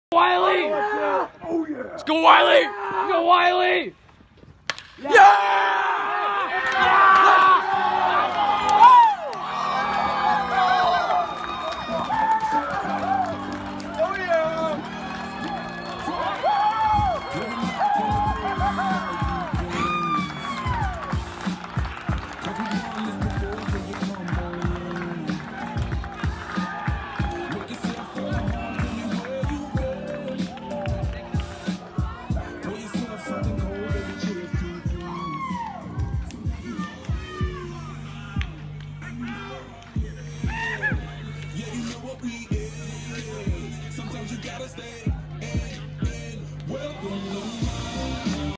2019 June - North Shore Junior Twins against Kent, Washington
Wylie Waters

Single walk off to get to the finals